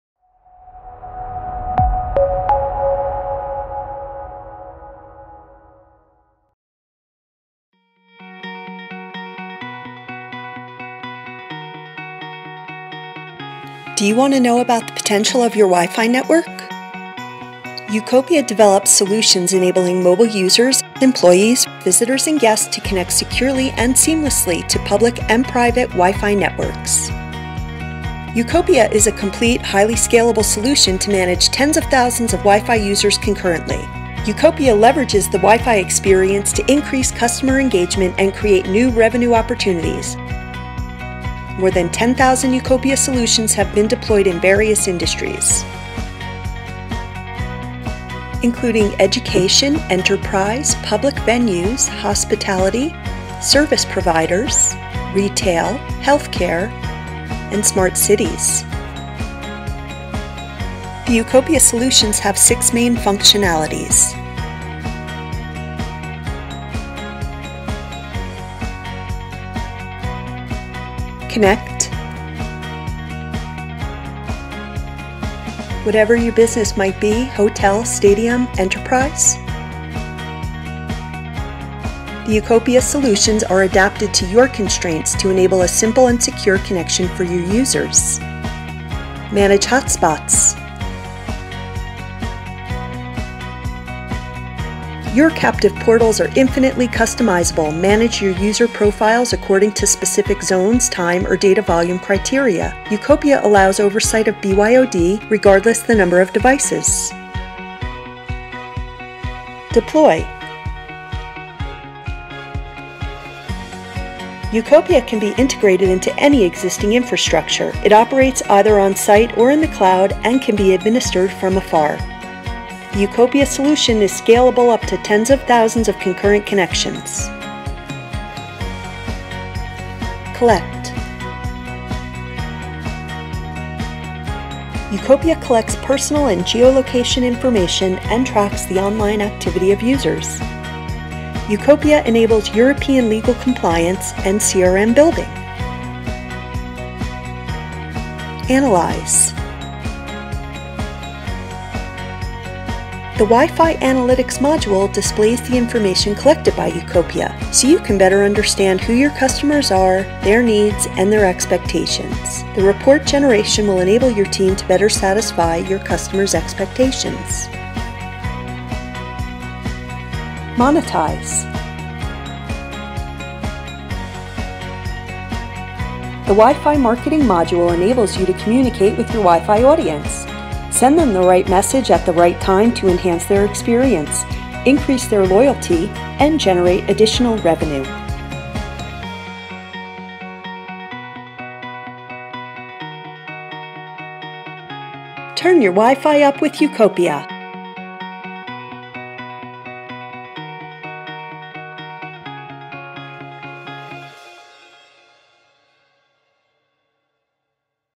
4Gon Solutions presents Ucopia - Turn up your WiFi
4Gon and UCOPIA can empower organisations to monetise their Wi-Fi infrastructure. We safely and securely connect visitors, guests, customers and audiences to any Wi-Fi network and we enable businesses, stadiums and cities to communicate with their users.

4Gon and UCOPIA connect organisations to their mobile users. We CONNECT our clients to their users, make hotspots MANAGEABLE, make services easy to DEPLOY, COLLECT information on users, ANALYSE how people use the wireless network, and help our clients MONETISE their Wi-Fi and ENGAGE with their customers.

Find out more by calling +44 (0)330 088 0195 today!